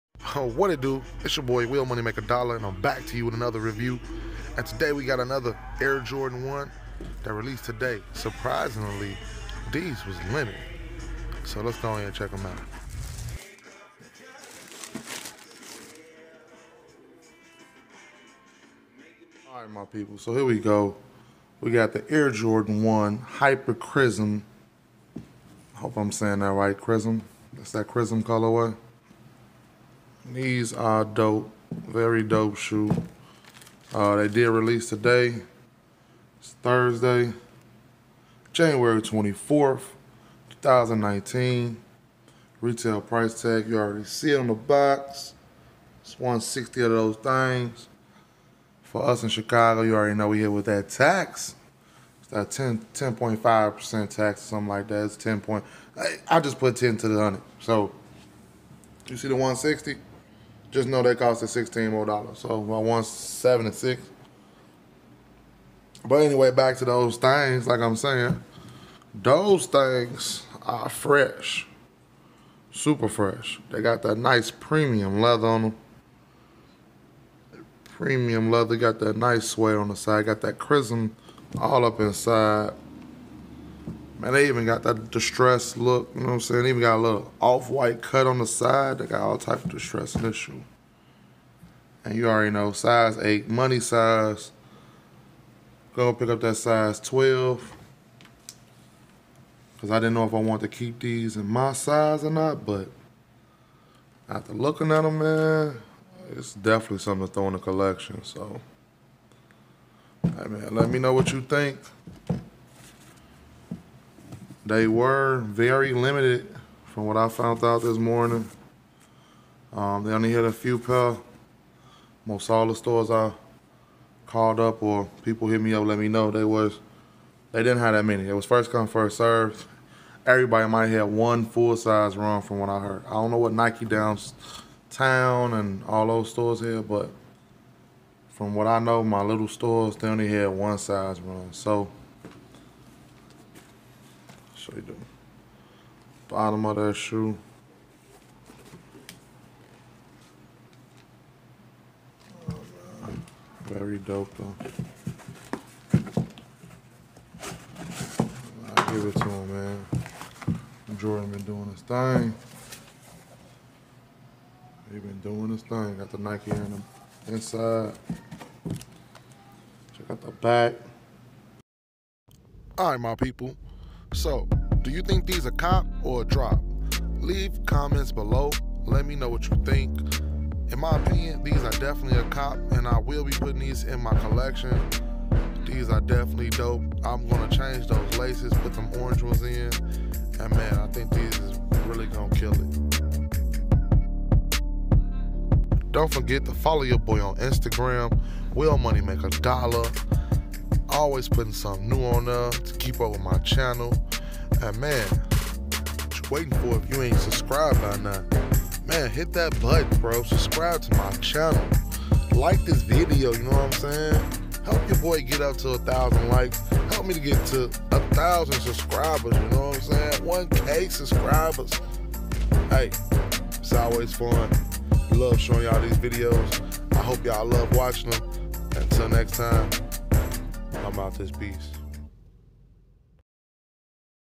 AIR JORDAN 1 "HYPER CRIMSON" AkA "Neutral Grey"
COME WITH ME AS I PICK UP THE NEW AIR JORDAN 1 OG "HYPER CRIMSON"